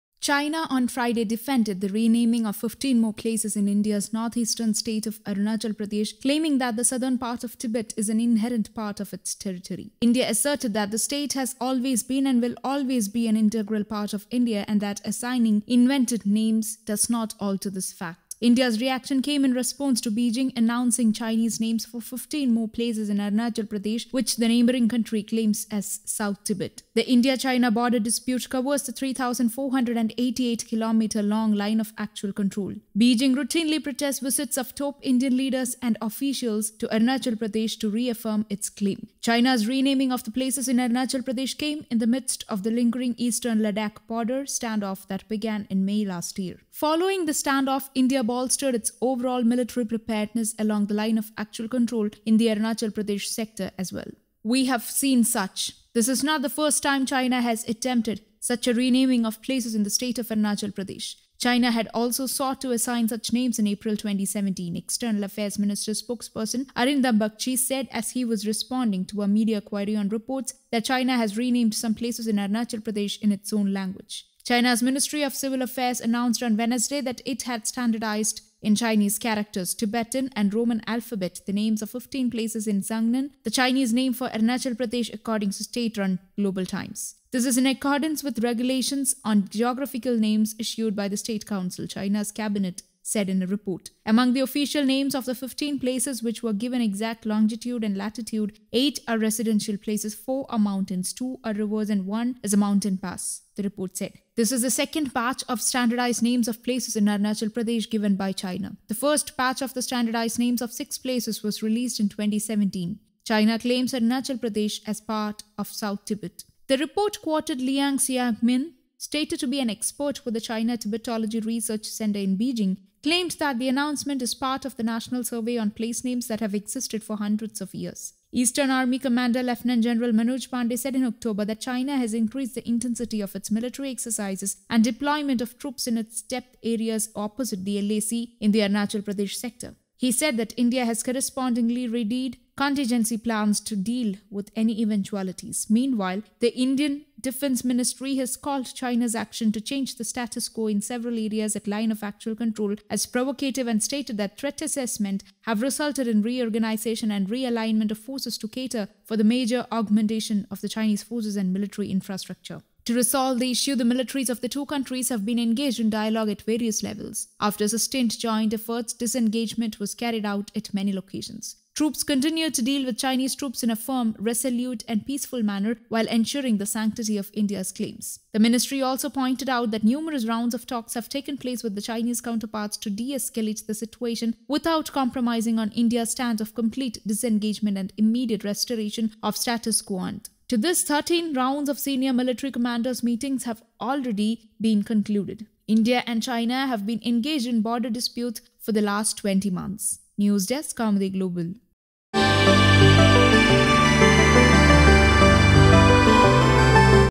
India-China tension flare up; China claims, renames 15 more places in Arunachal Pradesh
China on Friday defended the renaming of 15 more places in India's northeastern state of Arunachal Pradesh, claiming that the southern part of Tibet is an "inherent part" of its territory.
India asserted that the state has "always been" and will "always be" an integral part of India and that assigning "invented" names does not alter this fact. India's reaction came in response to Beijing announcing Chinese names for 15 more places in Arunachal Pradesh which the neighbouring country claims as South Tibet.
The India-China border dispute covers the 3,488 km long Line of Actual Control (LAC). Beijing routinely protests visits of top Indian leaders and officials to Arunachal Pradesh to reaffirm its claim.
China's renaming of the places in Arunachal Pradesh came in the midst of the lingering eastern Ladakh border standoff that began in May last year.
"We have seen such. This is not the first time China has attempted such a renaming of places in the state of Arunachal Pradesh. China had also sought to assign such names in April 2017," External Affairs Ministry spokesperson Arindam Bagchi said as he was responding to a media query on reports that China has renamed some places in Arunachal Pradesh in its own language.
China's Ministry of Civil Affairs announced on Wednesday that it had standardised in Chinese characters, Tibetan and Roman alphabet the names of 15 places in Zangnan, the Chinese name for Arunachal Pradesh, according to state-run Global Times.
This is in accordance with regulations on geographical names issued by the State Council, China's cabinet, it said in a report.
Among the official names of the 15 places, which were given exact longitude and latitude, eight are residential places, four are mountains, two are rivers and one is a mountain pass, the report said.
This is the second batch of standardised names of places in Arunachal Pradesh given by China.
The first batch of the standardised names of six places was released in 2017.
China claims Arunachal Pradesh as part of South Tibet.
The report quoted Lian Xiangmin, stated to be an expert with the China Tibetology Research Centre in Beijing, claiming that the announcement is part of the national survey on place names that have existed for hundreds of years.
Following the standoff, India bolstered its overall military preparedness along the Line of Actual Control (LAC) in the Arunachal Pradesh sector as well.

Eastern Army Commander Lt Gen Manoj Pande said in October that China has increased the intensity of its military exercises and deployment of troops in its depth areas opposite the LAC in the Arunachal Pradesh sector.
He said that India has correspondingly readied contingency plans to deal with any eventualities.
Meanwhile, the Indian Defence Ministry has called China's action to change the status quo in several areas at Line of Actual Control as "provocative" and stated that threat assessment have resulted in reorganisation and realignment of forces to cater for the major augmentation of the Chinese forces and military infrastructure.
To resolve the issue, the militaries of the two countries have been engaged in dialogue at various levels. After sustained joint efforts, disengagement was carried out at many locations.
Troops continue to deal with Chinese troops in a firm, resolute and peaceful manner while ensuring the sanctity of India's claims.
The ministry also pointed out that numerous rounds of talks have taken place with the Chinese counterparts to deescalate the situation without compromising on India's stand of 'Complete Disengagement and immediate Restoration of status quo ante'. To this effect, 13 rounds of senior military commanders' meetings have already been concluded.
India and China have been engaged in border disputes for the last 20 months.
(With inputs from PTI and IANS)